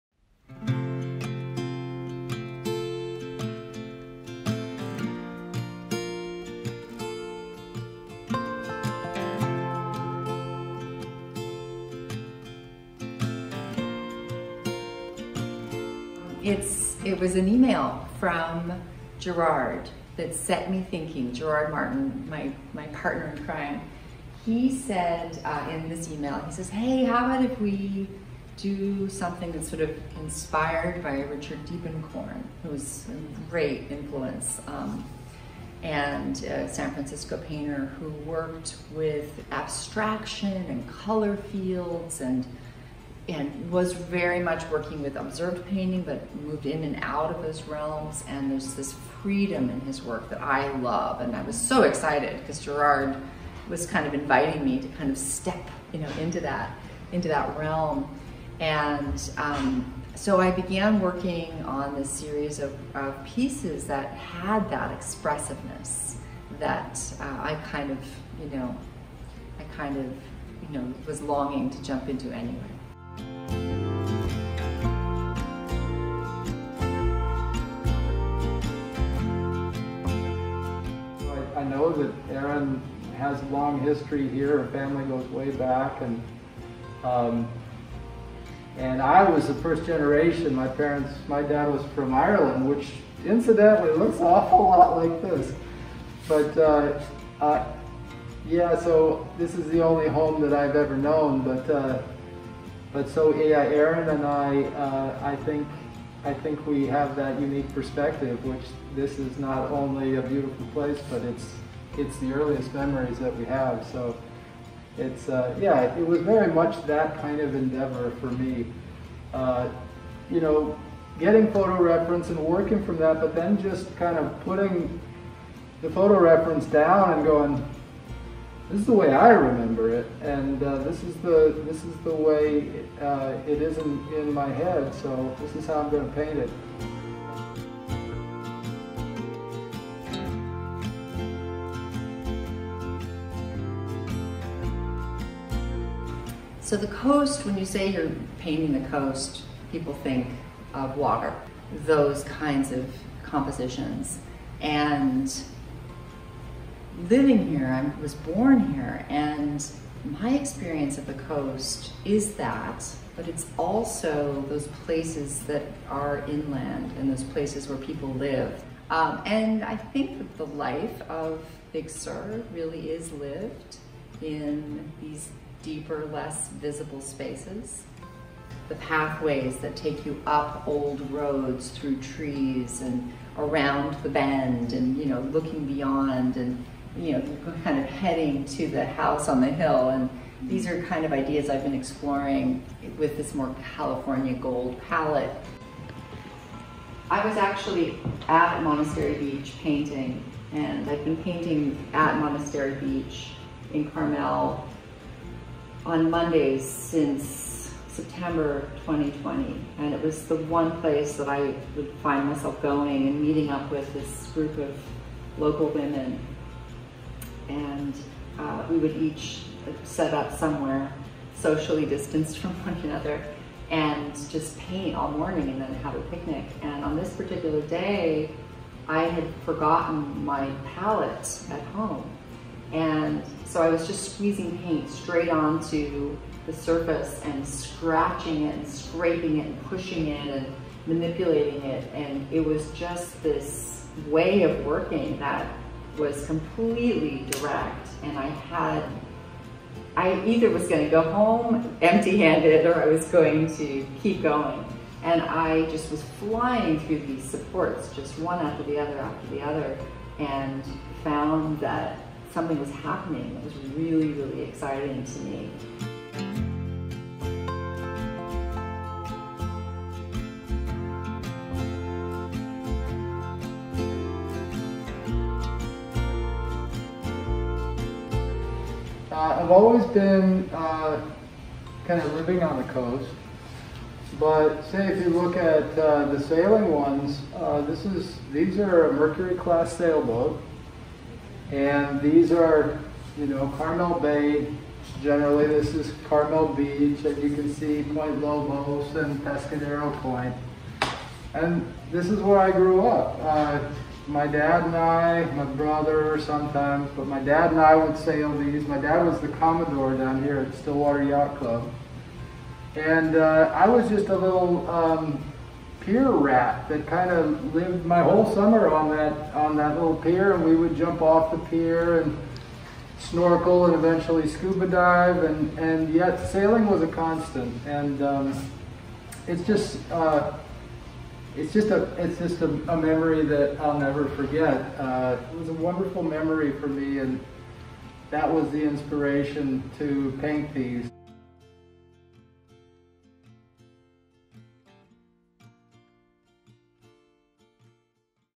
"Impressions/Expressions" Group Exhibition with Erin Lee Gafill and Gerard Martin
Gerard Martin and Erin Lee Gafill are known for their exuberant, color-rich oil paintings celebrating California’s Central Coast. The new oil paintings in this show weave their unique artistic influences into a triumphant whole.

Martin’s impressionist paintings of coast, harbor, sailboats, and the life of the bay share the wall with Gafill’s expressionist works that investigate coastline, mountainside, and valley. Both use strong color to convey emotion, connection, and portray a unique sense of place.